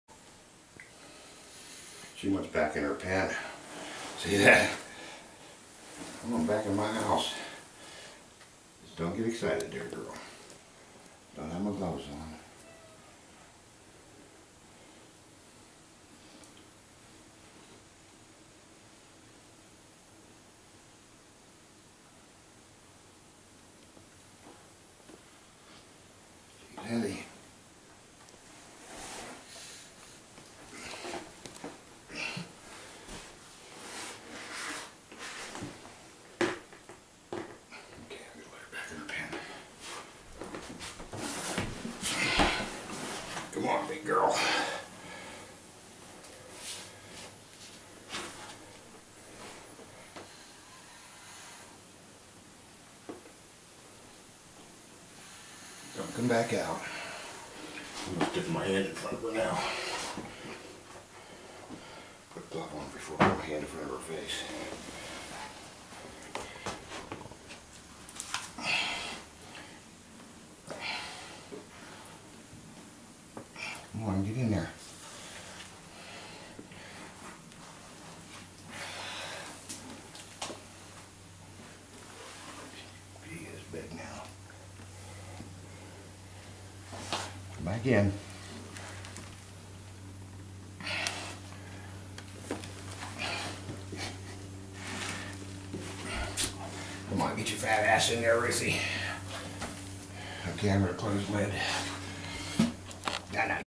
Burmese Python 02
This is me with Ruthie, our female Burmese Python.